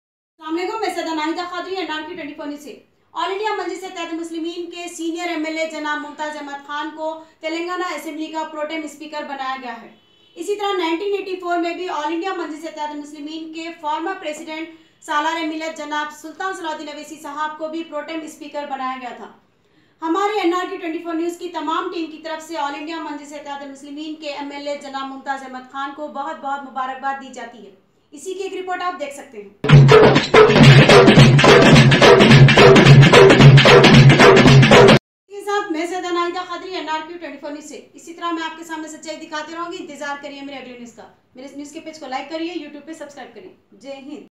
AIMIM Senior MLA Janab Mumtaz Ahmed Khan ko Telangana Assembly ka Pro-tem Speaker
AIMIM Senior MLA Janab Mumtaz Ahmed Khan ko Telangana Assembly ka Pro-tem Speaker Banaya Gaya hai... Pehle bhi 1984 mei AIMIM Former President Salar-e-Millat Janab Sultan Salahuddin Owaisi Sahab ko bhi Pro-Tem Speaker Banaya gaya tha...
Hamare NRQ 24 News team ki taraf se AIMIM MLA Janab Mumtaz Ahmed khan ko Mubarakbaad dijati hai..